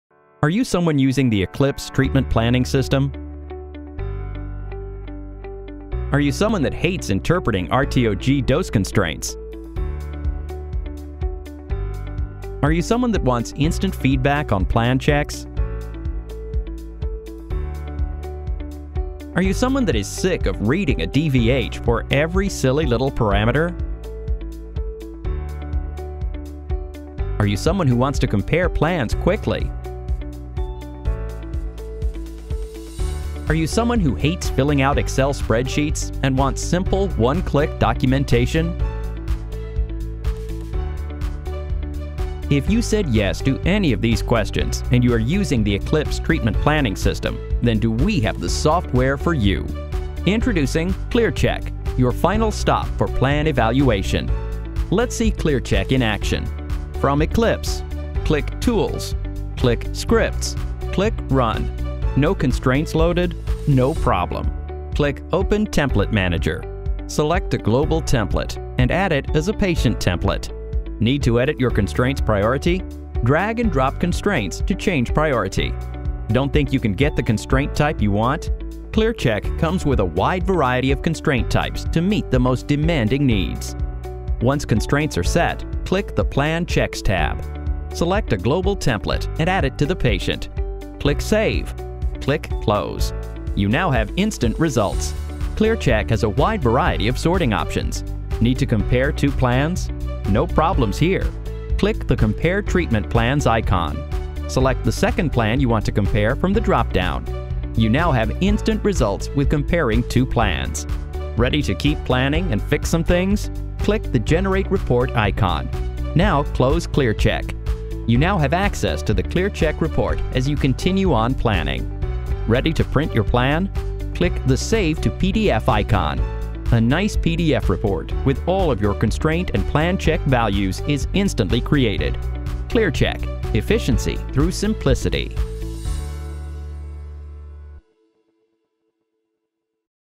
ClearCheck
ClearCheck is plan evaluation software used for the Eclipse Treatment Planning System.  ClearCheck seamlessly integrates with Eclipse to evaluate patient dose constraints and plan checks.  Patient documentation is easily created in the click of a button.